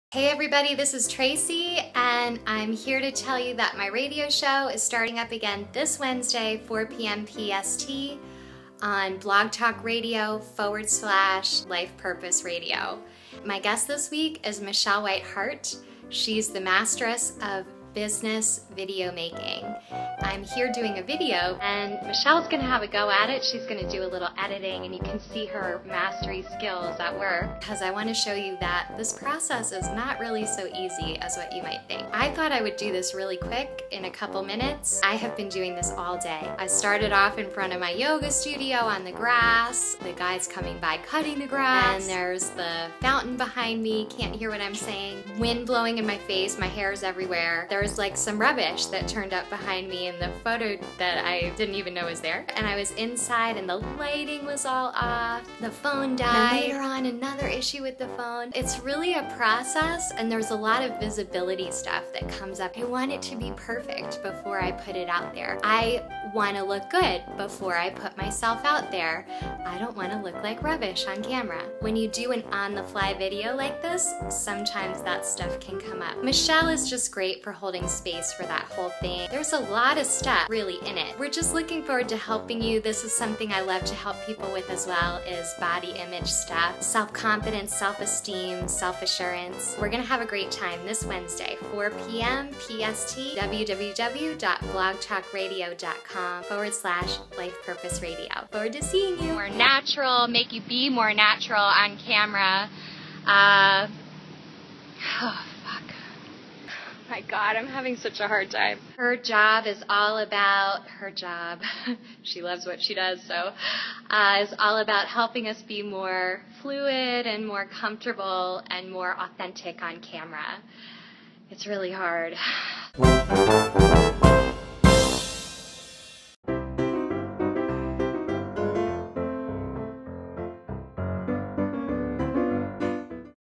"Gettin' Over Visibility Issues to Make Your Youtube Videos" Tracy McDonald Interviews Michelle Hart
Tracy McDonald interviews Michelle Hart - Video Visibility Coach- about "Getting over Your Visibility Issues to Make Your Youtube Videos" Wednesday August 7th 4 pm PST.

to learn more about Tracy McDonald
and
to learn more about Michelle Hart